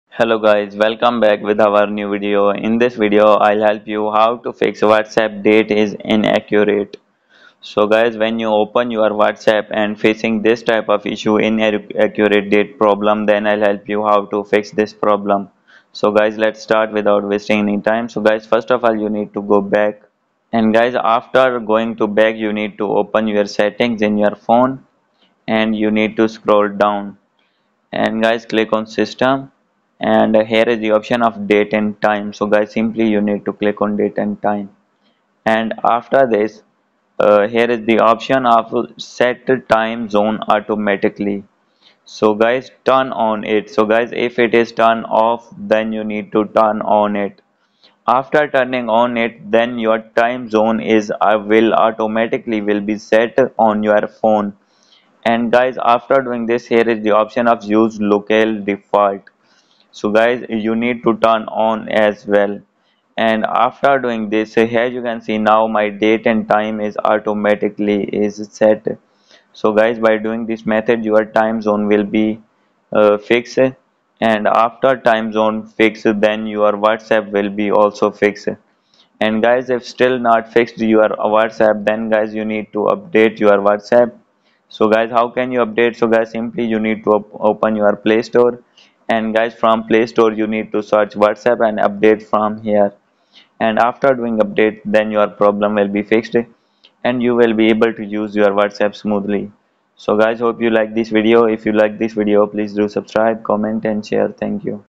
How To Fix WhatsApp Date is Inaccurate | WhatsApp Date And Time Problem Fix
In this video, I will help you to fix WhatsApp Date is Inaccurate. Please watch till the end.